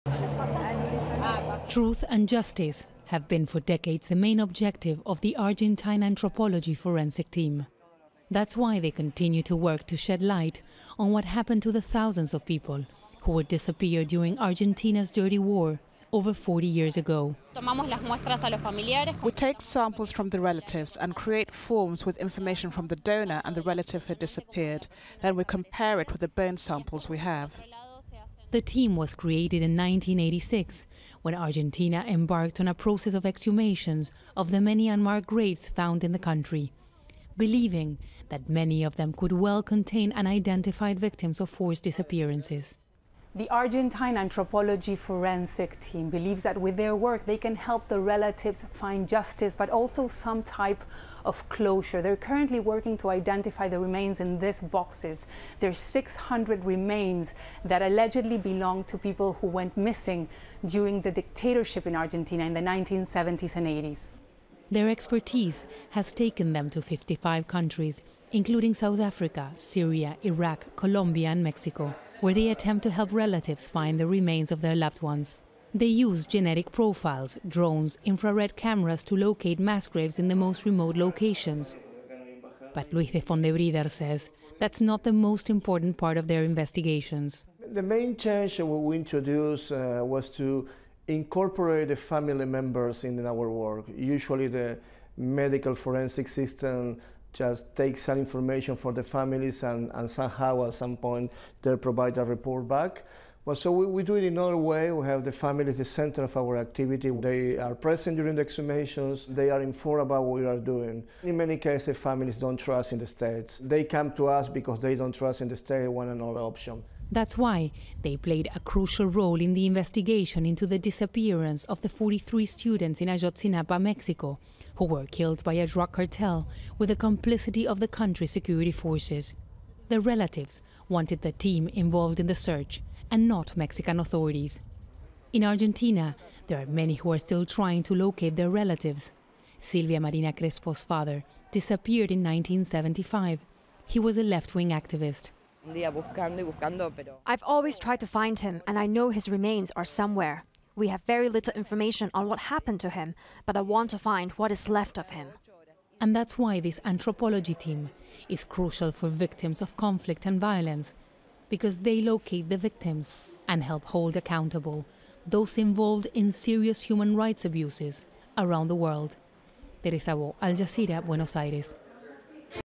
Argentina forensics team travels the world to identify missing
A team of forensic scientists in Argentina is helping to bring peace to families who lost their loved ones in the so-called "Dirty War".
That was a period in the 1970s and '80s when government death squads hunted down and killed political opponents.
The work to identify those who disappeared is ongoing and scientists are lending the skills they developed to other conflict areas across the globe.

Al Jazeera's Teresa Bo reports from Buenos Aires.